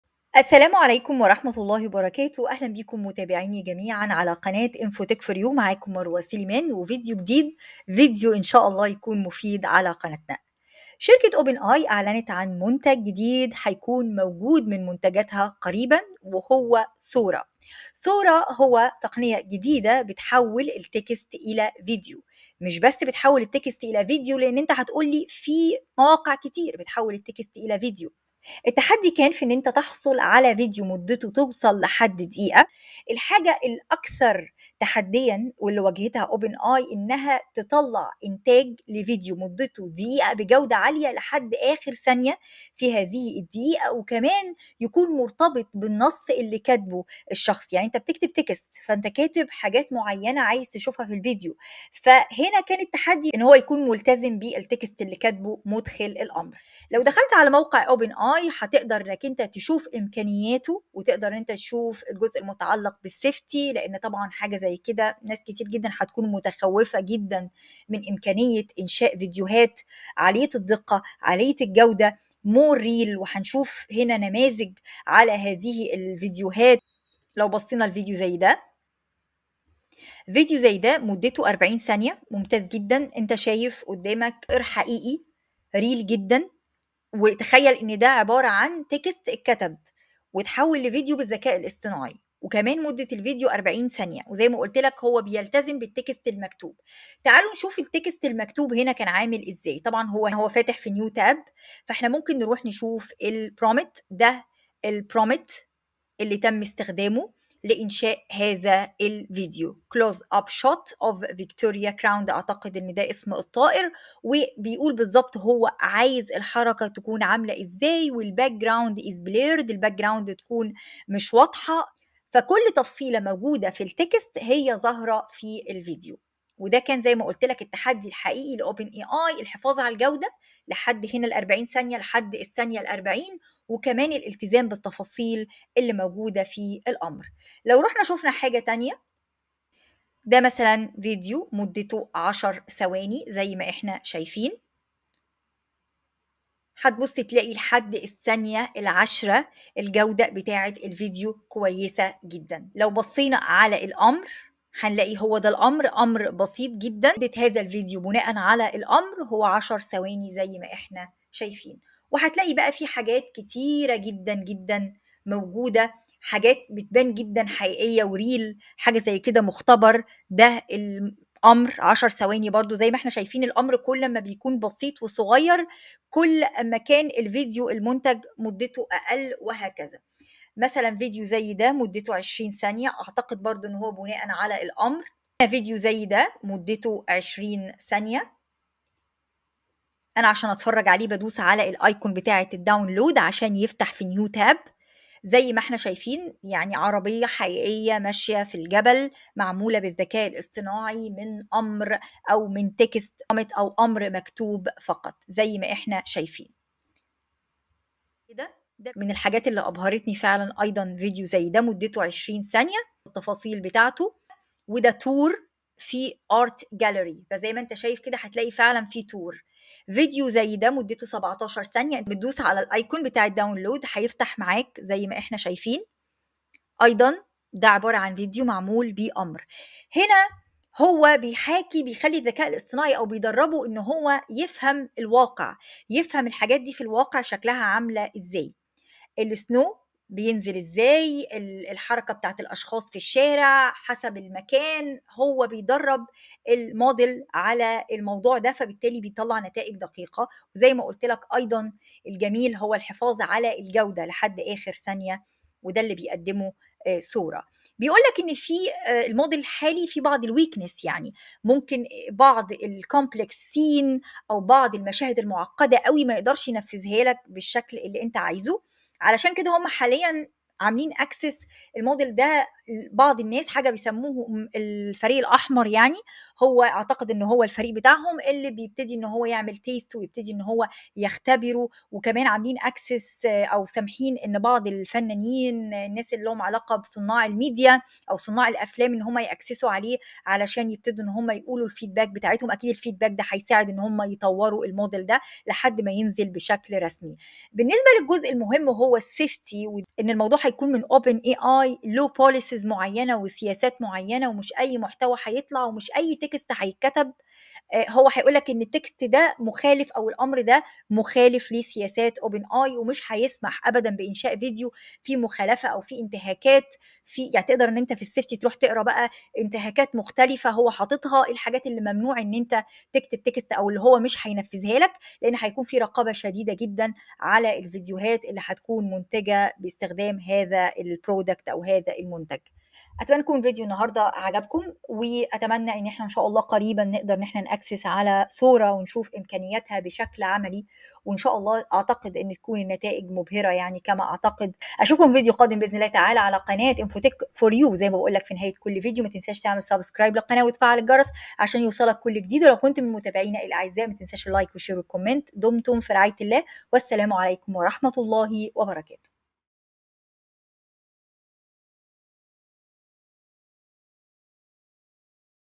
Sora from Open AI | 🔥🔥 الذكاء الاصطناعي فاق الحدود
شركة open ai اعلنت عن منتج سوف يصدر قريبا وهو sora تعرف على هذا المنتج ومميزاته
المصدر: